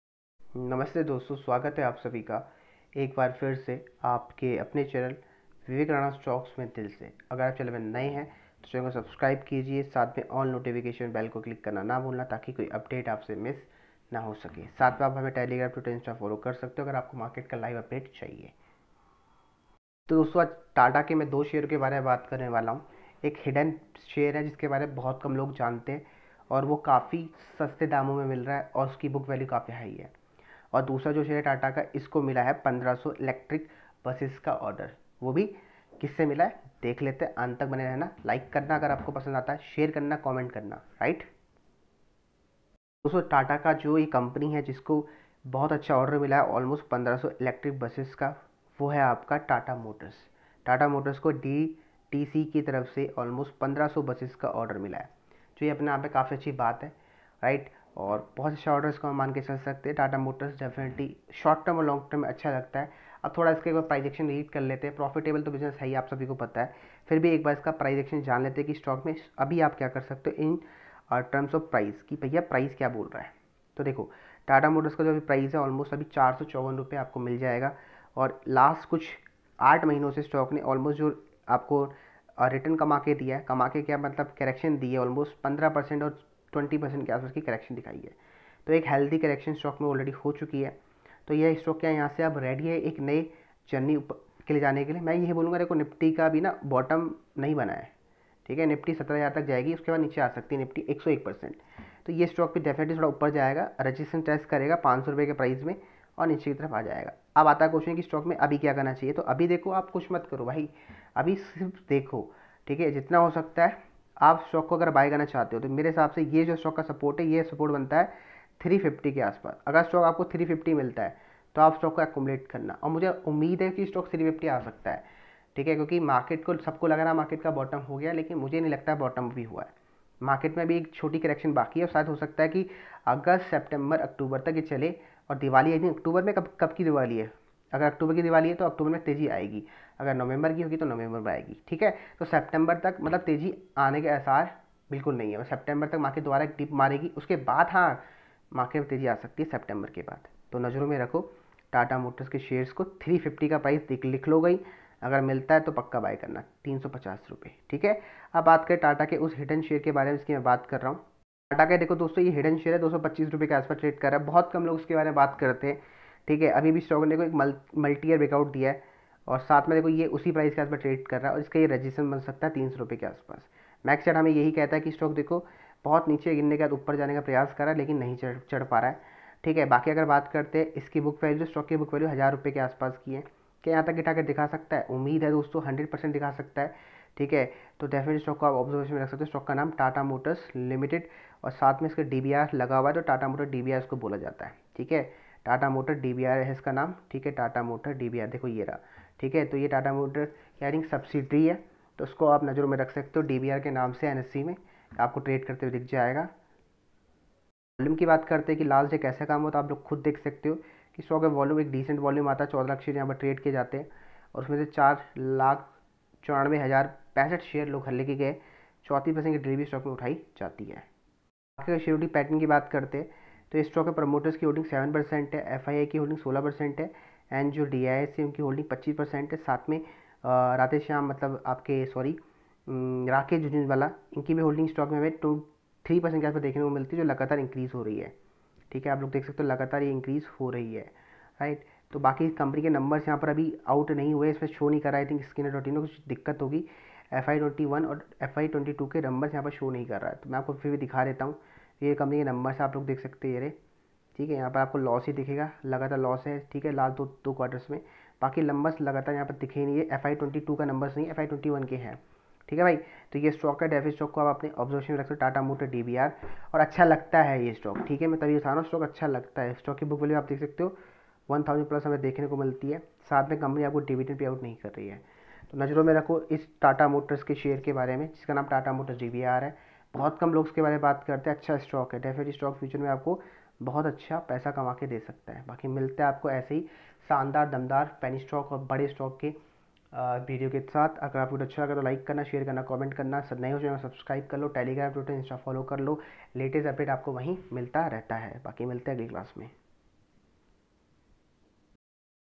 TATA KE SHARE KO MILA BADA ORDER 1500 ELECTRIC BUSES – TATA KA HIDDEN PENNY STOCK – Book value 1086
TY :)